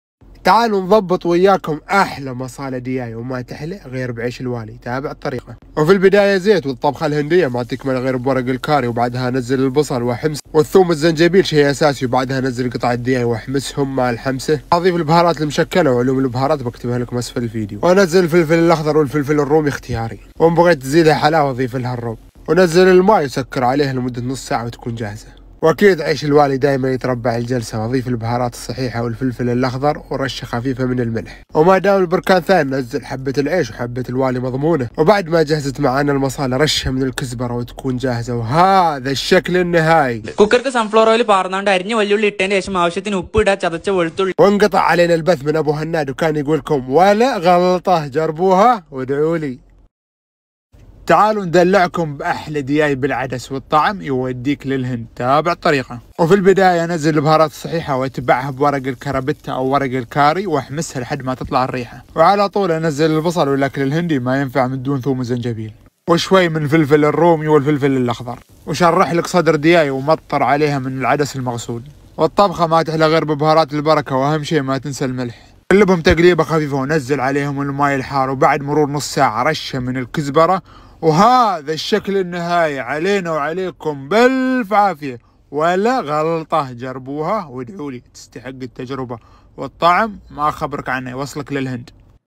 مسالا دجاج هندي+دجاج بالعدس
مسالا دجاج هندي

المكونات⚠️
دجاج 🍗
بصل🧅
ثوم 🧄
زنجبيل⚜️
كزبرة ☘️
روب🍦
ورق كاري🍃
فلفل اخضر🌶
فلفل رومي🫑
عيش 🌾

البهارات⚠️
فلفل اسود-فلفل احمر-بابريكا-كزبرة-كمون-بهارات المسالا- كاري

الطريقة⚠️
في البداية نحمس ورق الكاري مع البصل الثوم والزنجبيل واضيف الدجاج واحمسه والفلفل والبهارات والملح والروب واحمسهم وانزل الماي الحار ولمدة نص ساعه وفي الاخير رشة من الكزبرة وبالعافية

دجاج بالعدس
الله على الطعم الي يوصلك للهند😁

المكونات ⚠️
عدس🥜
صدر دجاج🍗
بصل🧅
فلفل رومي🫑
فلفل حار🌶
ثوم🧄
زنجبيل⚜️
كزبرة🍃
ورق كاري☘️

البهارات البركة ⚠️
كاري - فلفل اسود - كمون - بابريكا - كزبرة - بهارات الدجاج

الطريقة ⚠️
نحمس البهارات الصحيحه مع ورق الكاري ونضيف البصل ونحمس وبعدها نضيف الثوم والزنجبيل والفلفل الرومي والحار ونضيف صدور الدجاج ونحمس وبعدها نضيف العدس والبهارات ونحمسهم ونضيف الماي الساخن ونتركة نص ساعه وفي الاخير رشة من الكزبرة وبالعافية